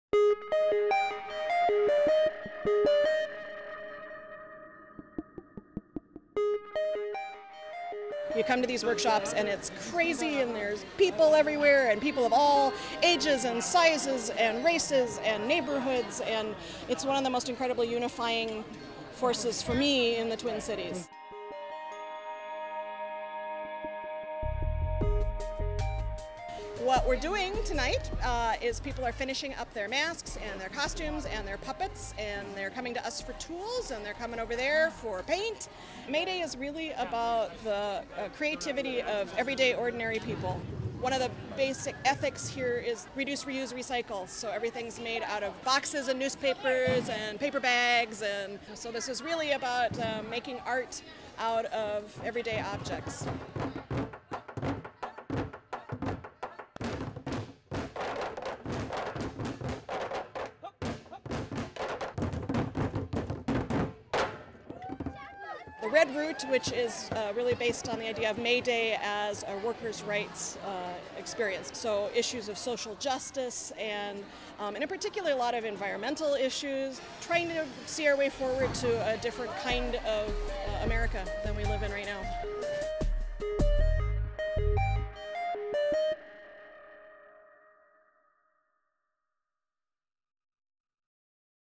MN2020: In The Heart of the Beast
The 38th annual May Day Parade and Festival put on by the In the Heart of the Beast Puppet and Mask Theater is a Minnesota staple and nationally recognized a model for community arts initiatives.
In addition to visually stunning theater, the event brings attention to issues of food justice, water use, transportation, and energy policy solutions
This year, Heart of the Beast is teaming up with Blooming Towns to foster discussion and action beyond the parade and festival.